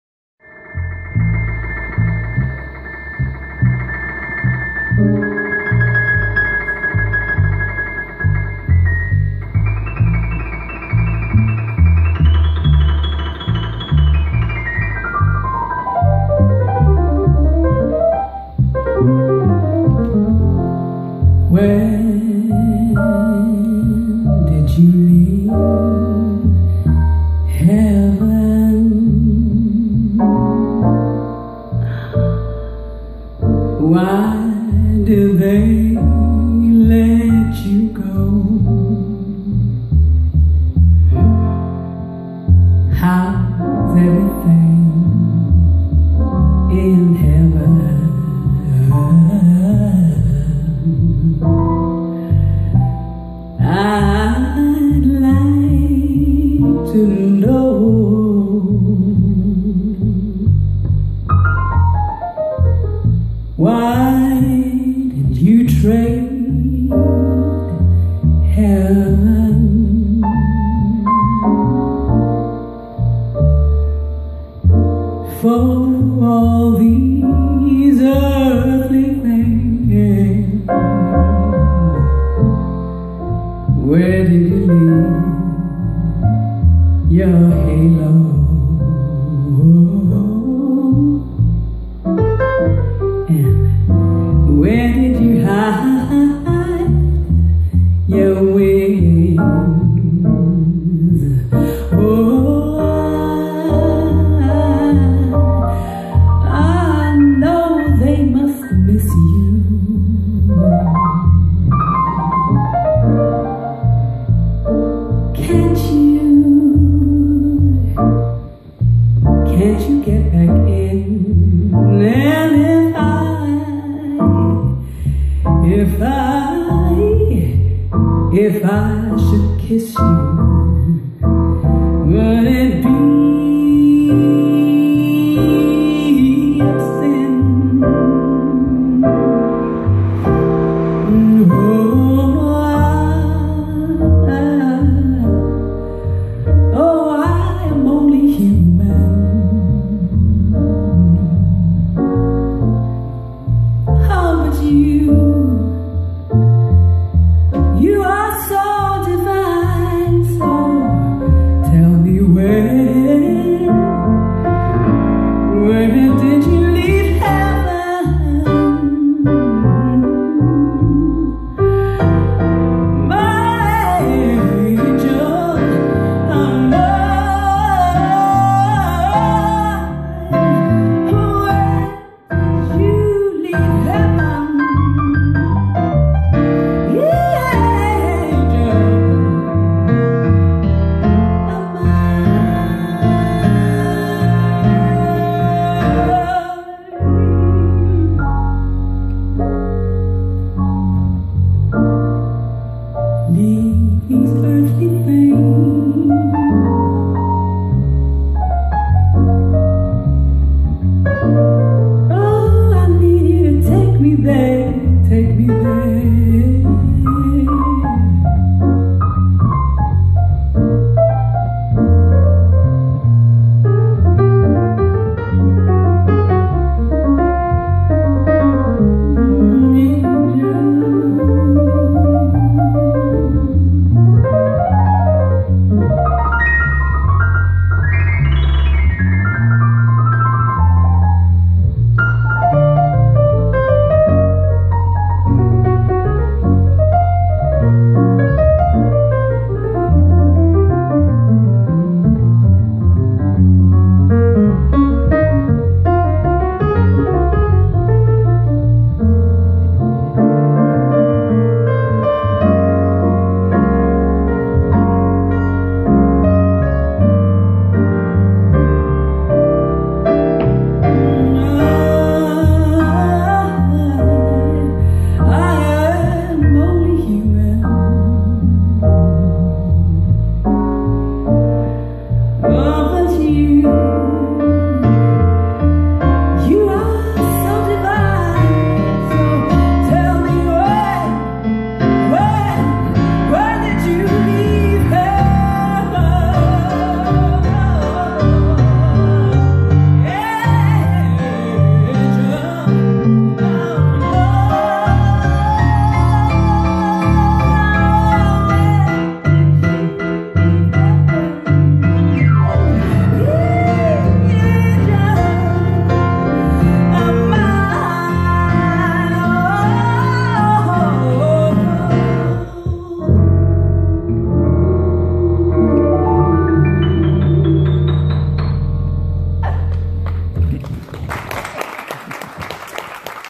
Sharon Dyall & Tommy Berndtsson @ Jazzens Museum (2015) #1
Sharon Dyall: vocals
Tommy Berndtsson: piano, keyboards
Lars Ericsson: bass